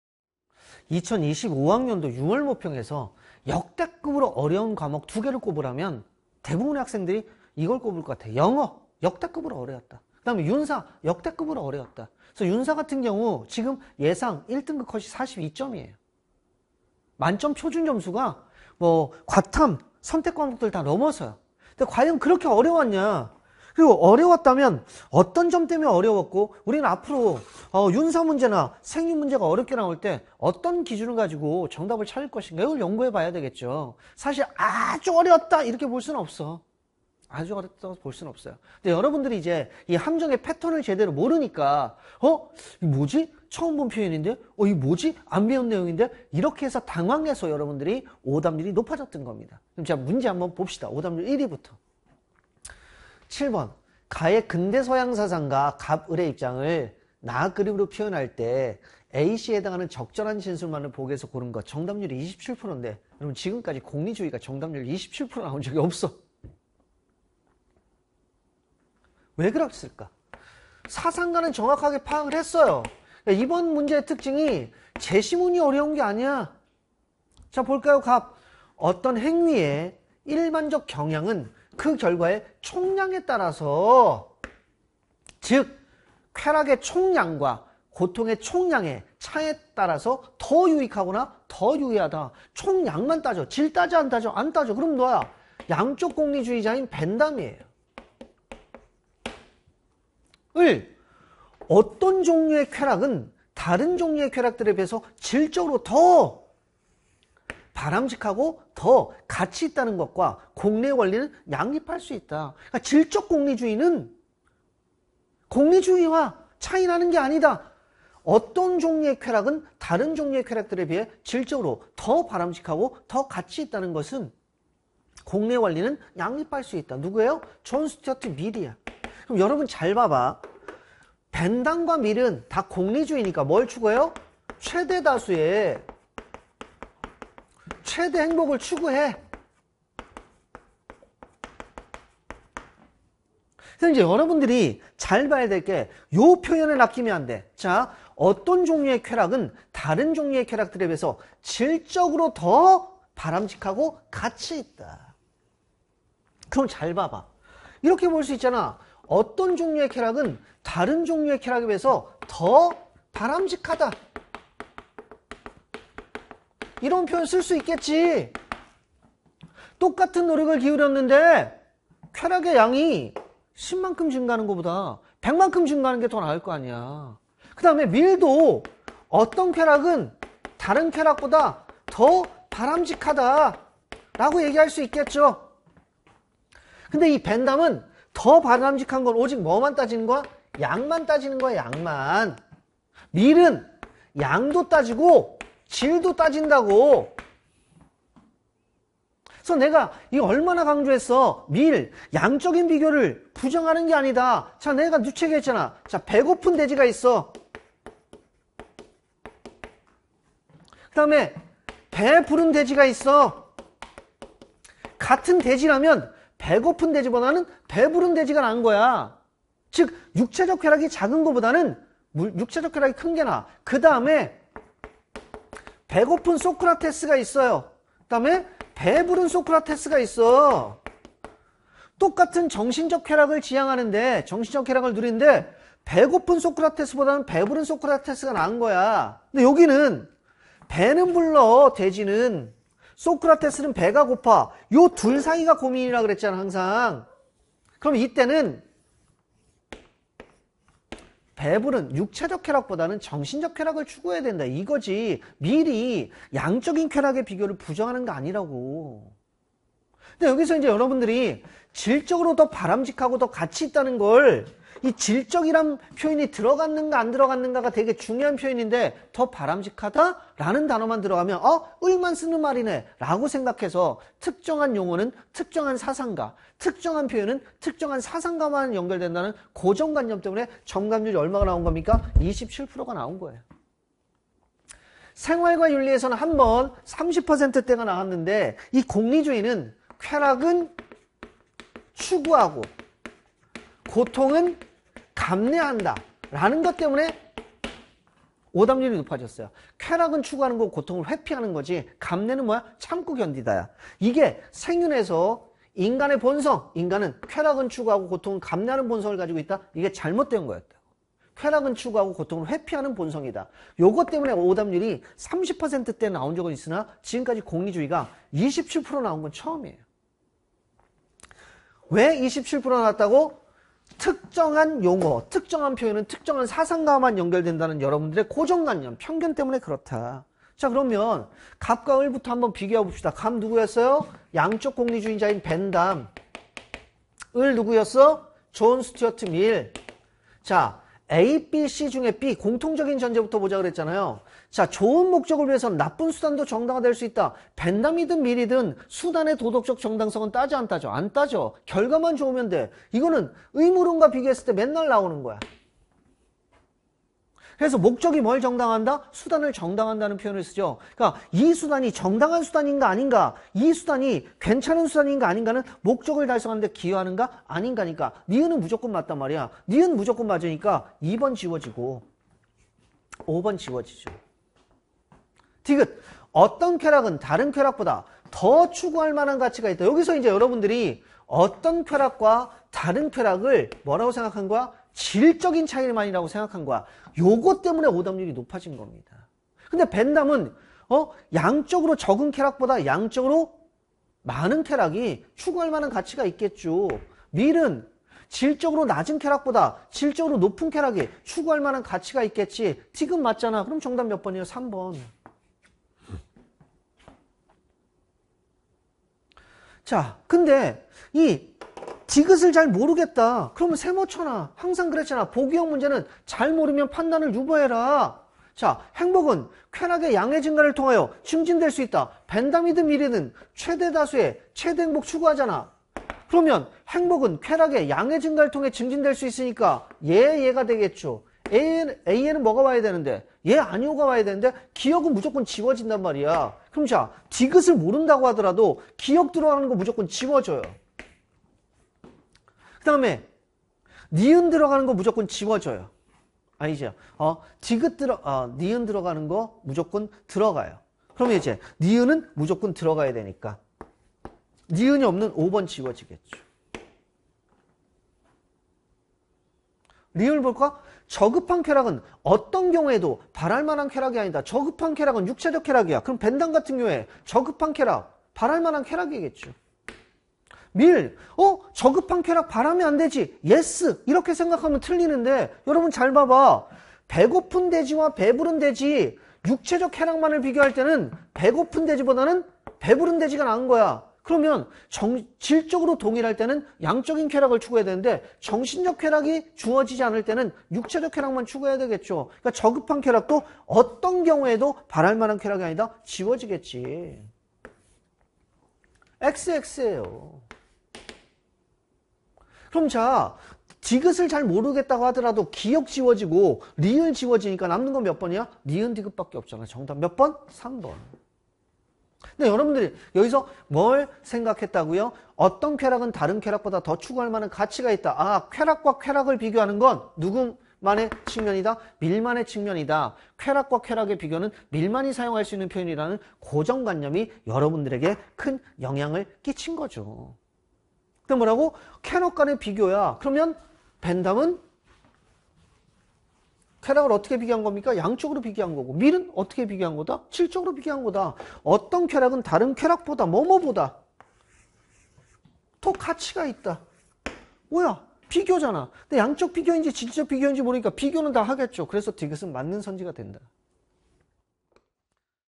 [메가스터디] 윤리 강상식쌤 - 2025학년도 고3 6월 모평 윤리와사상 해설강의
윤리 강상식 선생님의 2025학년도 고3 6월 모평 윤리와사상 해설강의 영상입니다.

- 윤리 강상식 선생님을 더 알고 싶다면?